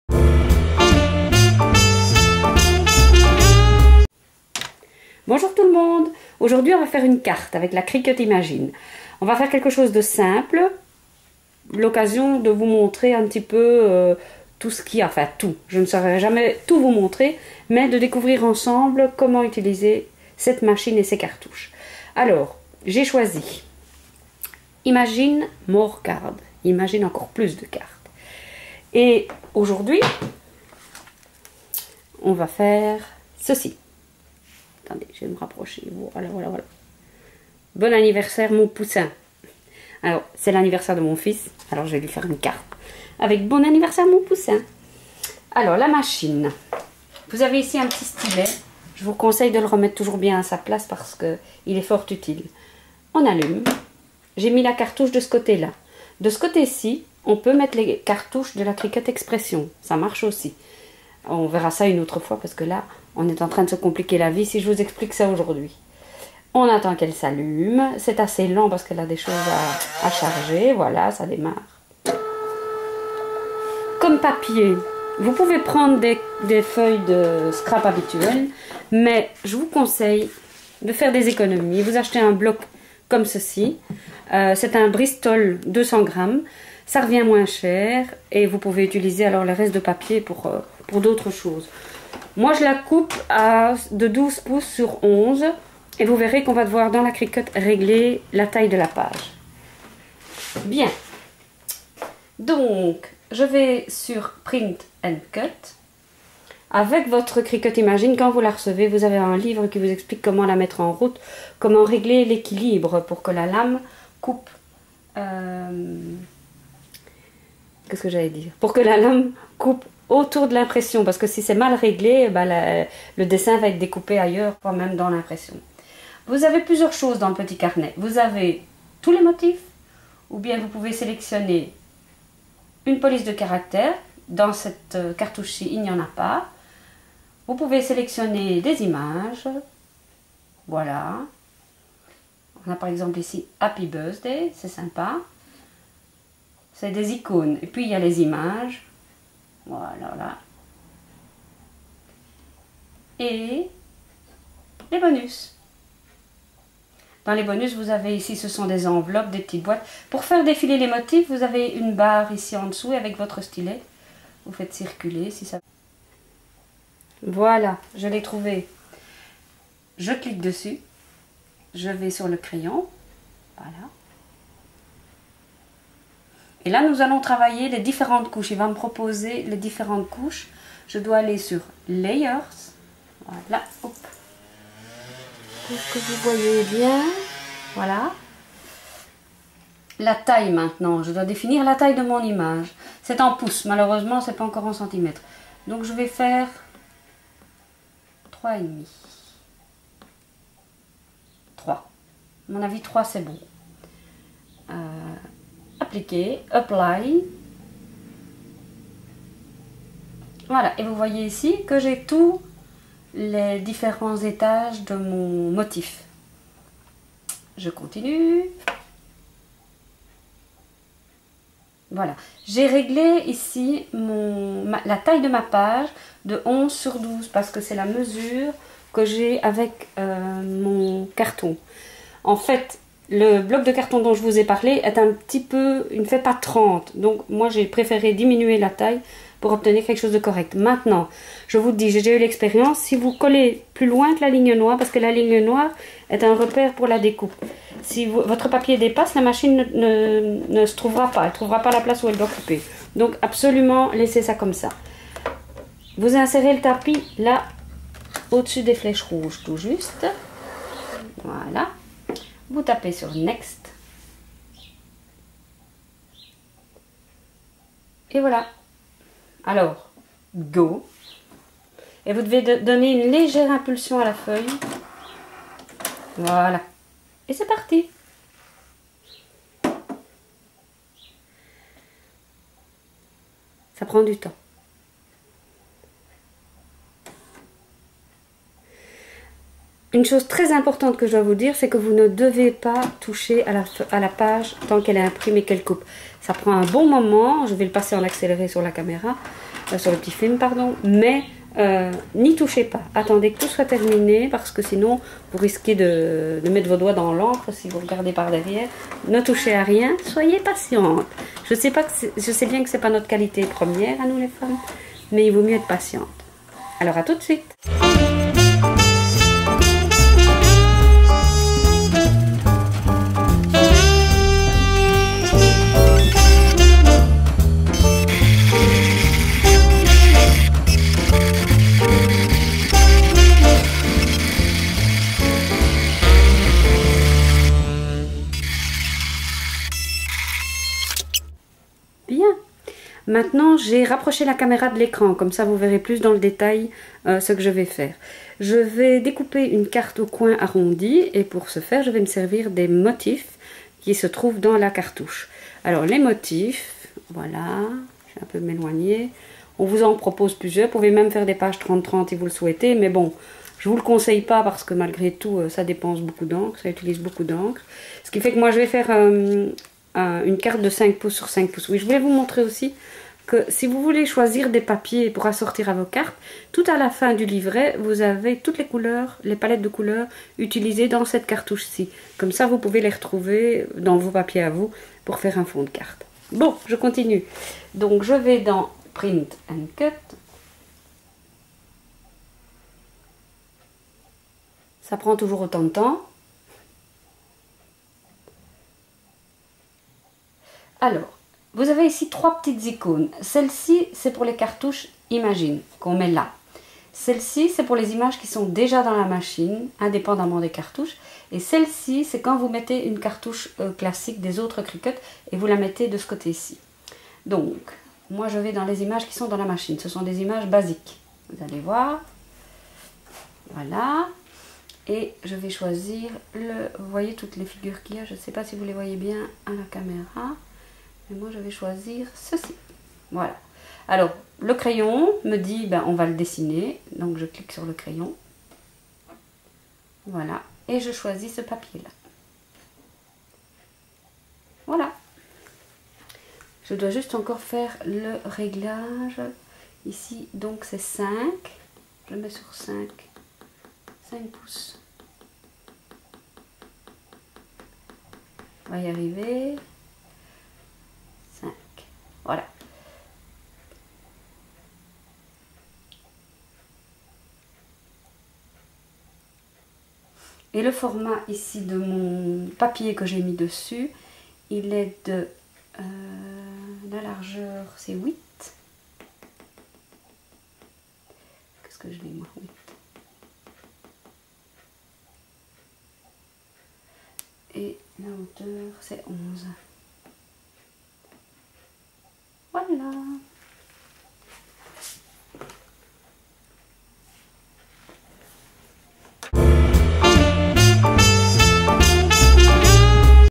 carte cricut imagine Happy Birthday : tuto en français
Enfin, en français des vidéos pour la Cricut Imagine. Comment débuter, comment s'en sortir ?
N'hésitez pas à me poser des questions...